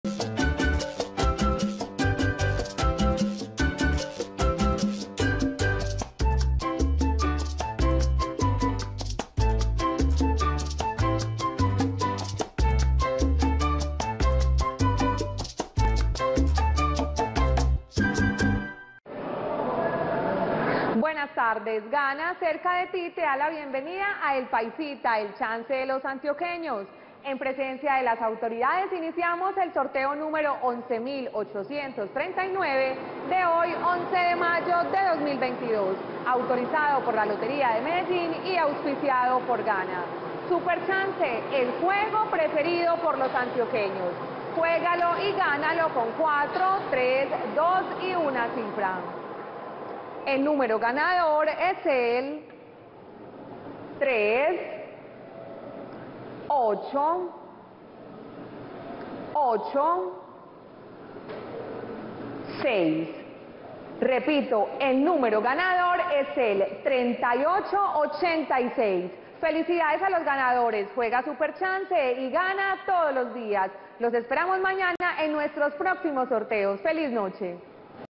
EL PAISITA 2 Resultados del Chance del MIÉRCOLES 11 DE MAYO de 2022 ✅🥇🔥💰
Hola buenas noches, te presentamos los resultados de las loterías y chances en Colombia, vamos con el Resultados del Chance El Paisita 2 del MIÉRCOLES 11 DE MAYO de 2022.